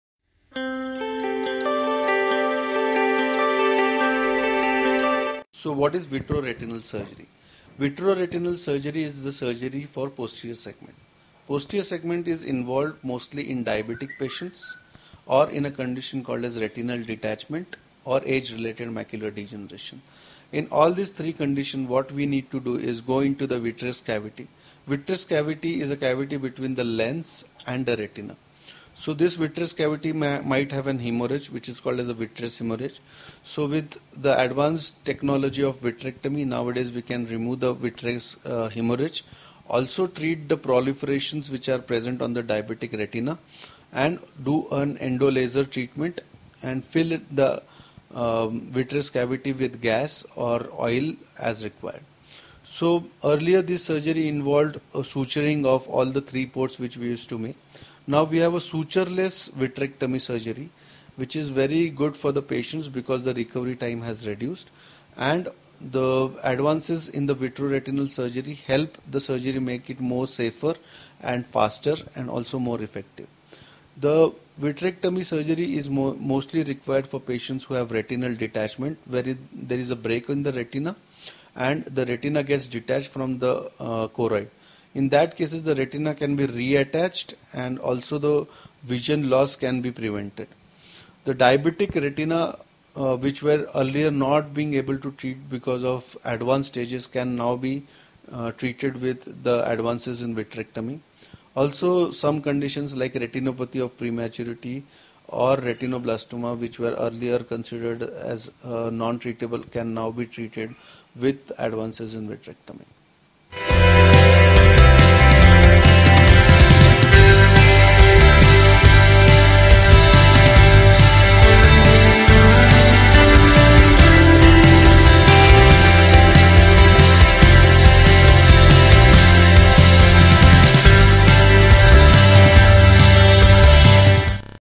Vitreoretinal Surgery - Treatment Option for Eye Problems
To repair or fix different eye conditions such as, retinoblastoma, diabetic retina, age related macular degeneration or ratiopathy of prematurity Vitreoretinal surgery is performed. Watch as Dr. Anand describes about vitrectomy, suture-less vitrectomy surgery and how these types of surgeries are safer, faster and more effective. Watch!